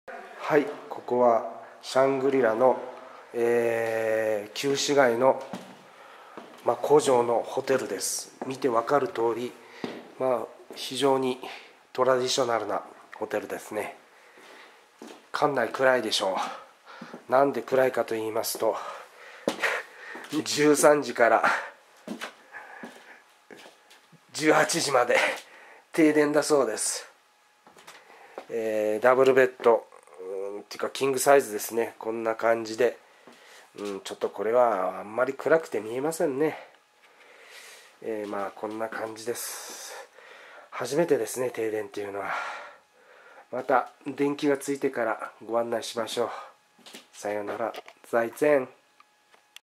雲南省、香格里拉（シャグリラ）のホテル
標高３３００ｍの高地のため、高山病気味。

しかもホテルは１８時まで停電中( 一一)

それでも、スタッフのお兄さんは優しげで親切！